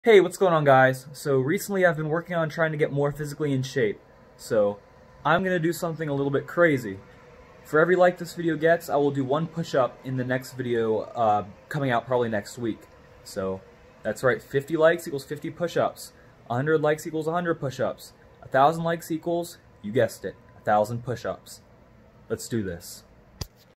1 LIKE=1 PUSH-UP
and for every comment ill do a sit-up!!!

Yes, you read that title correctly...
 I've been working on trying to get more in shape physically, so for the next video, I will be doing as many push-ups as this video gets likes.